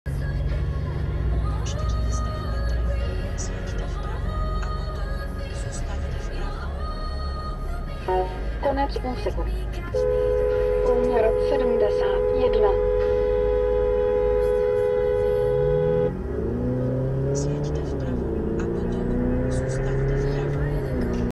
jebat se vlevo a 50kmh, to by mu šlo...
Dědek plesnivej ještě ukazoval, ať jedu prostředkem.
a) jezdí se vpravo - to je základ
b) v tom místě je 80kmh, takže padesátkou by opravdu stálo za to, jet více vpravo